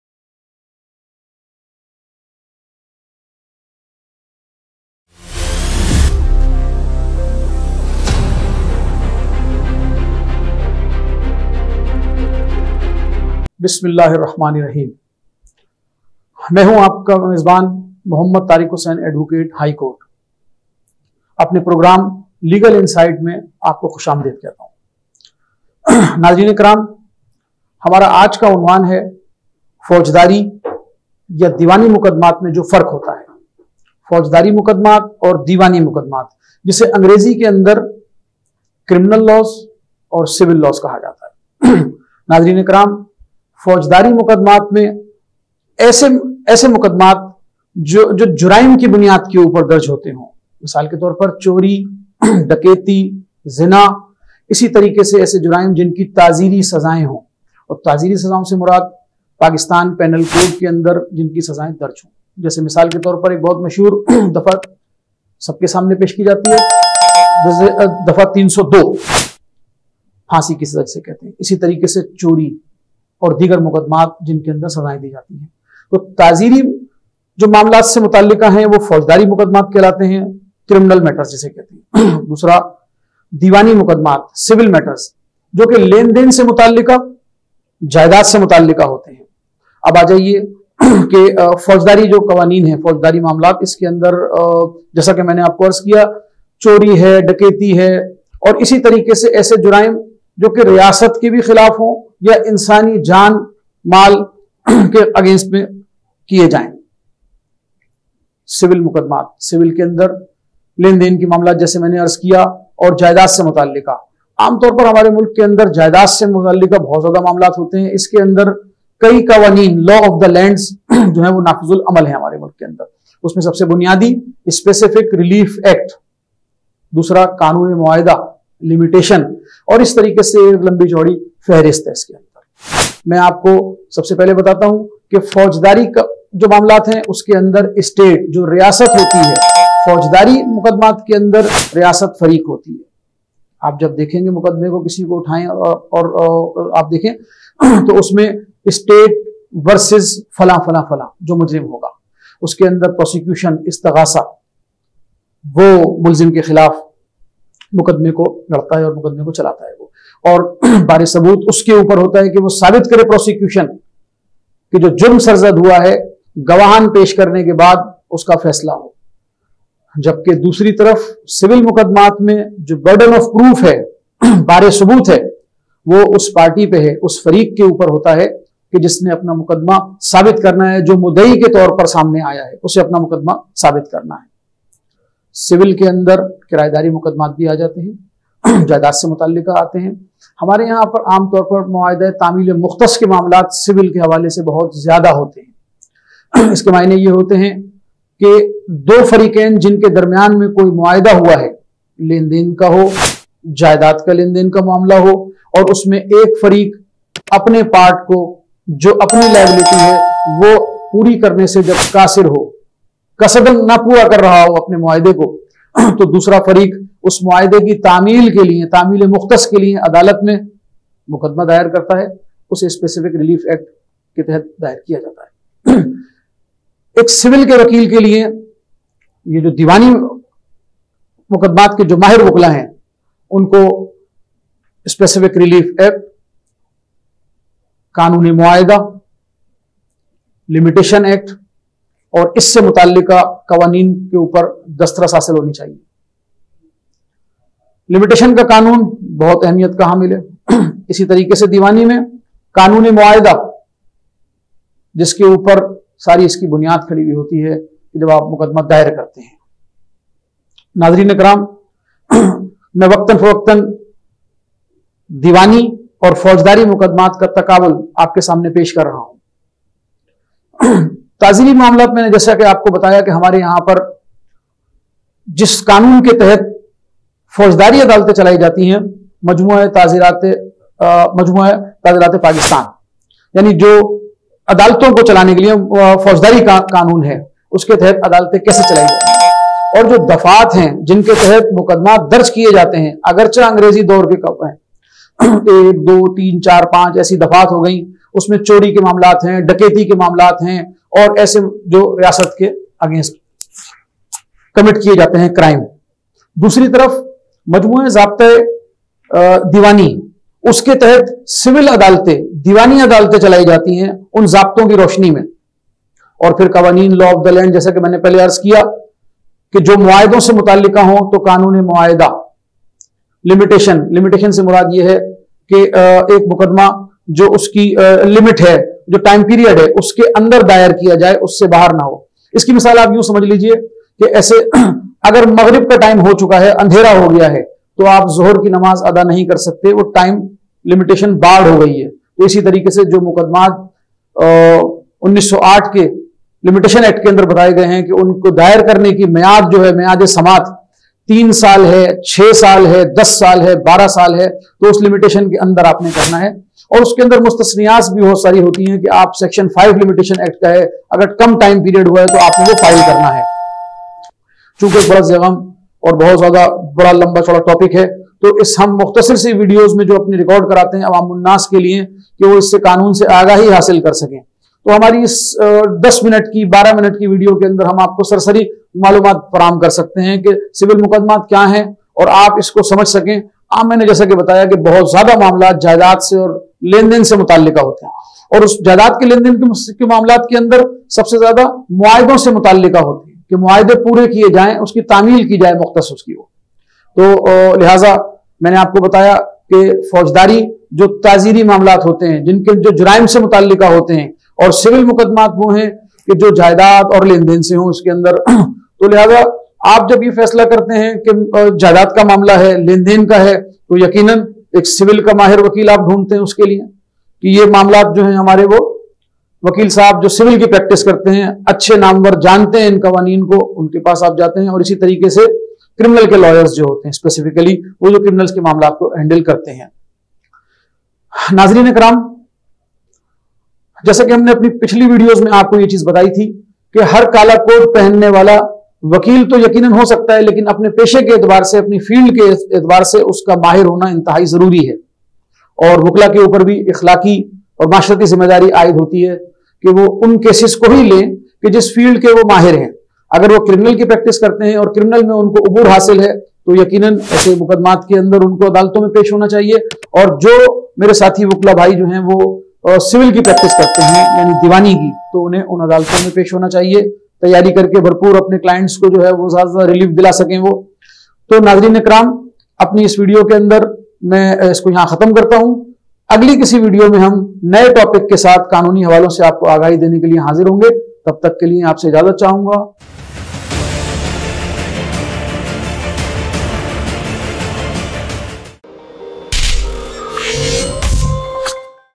Criminal and Civil Law|| By Muhammad Tariq Hussain|| 9T9 News
Criminal law is concerned with offenses against the state or society. Its primary purpose is to punish individuals who have committed crimes as a means of maintaining public order and safety.

Civil law deals with disputes between private parties and aims to provide remedies and compensation to the injured party rather than punishment.